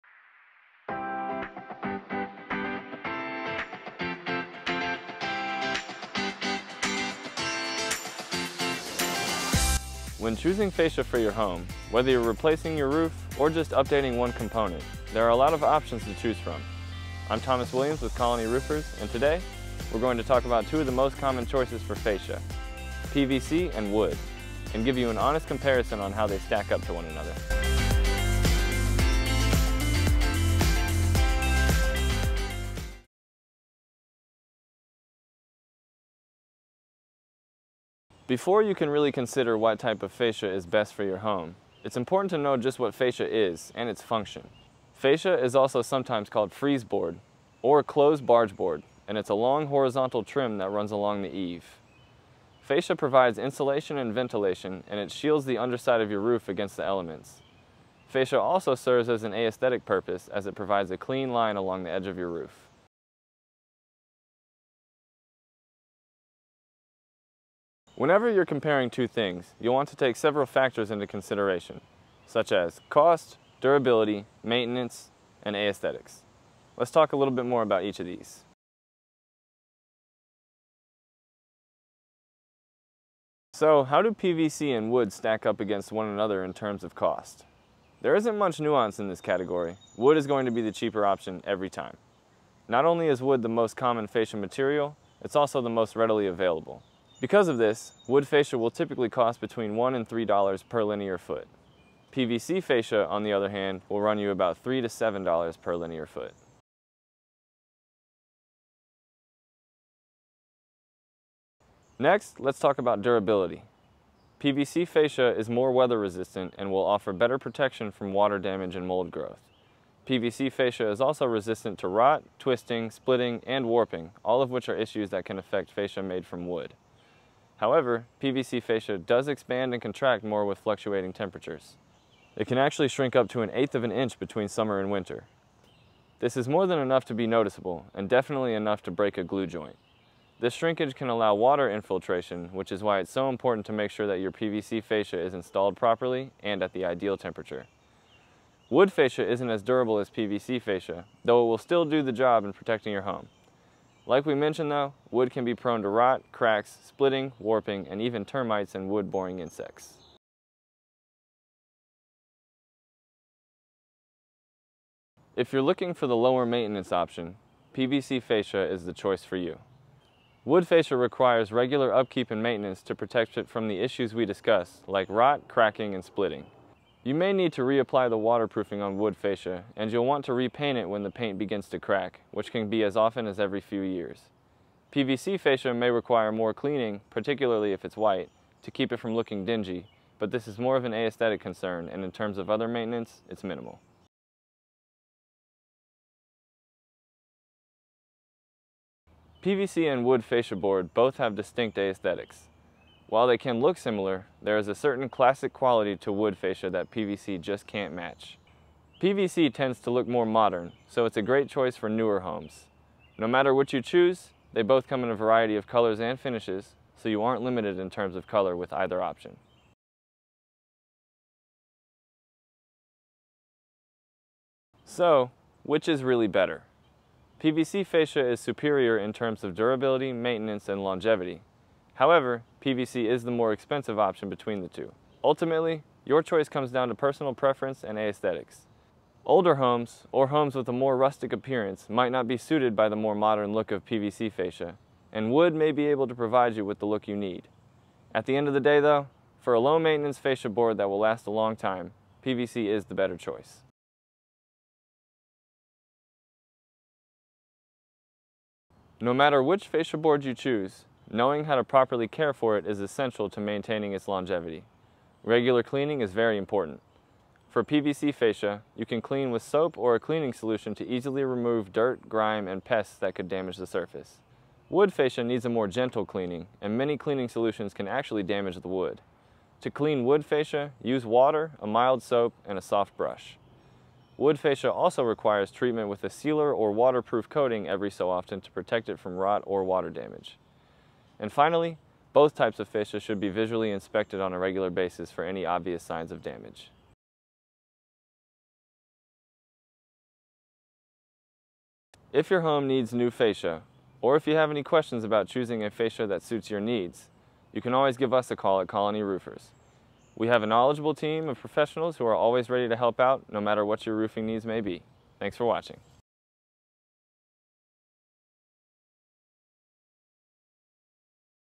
PVC vs Wood Fascia - What Is The Best Kind Of Fascia Board?
Are you wondering about the different types of fascia, and what kind of fascia is the best? In this video we take a look at both wood fascia and PVC fascia, and discuss what fascia boards might work best for your needs.

715 Peachtree St. NE, Suite 100
Atlanta, GA 30308
00:00 Introduction
00:34 What Is Fascia?
01:03 PVC vs Wood Fascia
01:21 Cost
01:49 Durability
02:43 Maintenance
03:16 Aesthetics
03:40 Which Is Better?
04:16 Tips For Fascia Care
04:57 Need Your Fascia Replaced?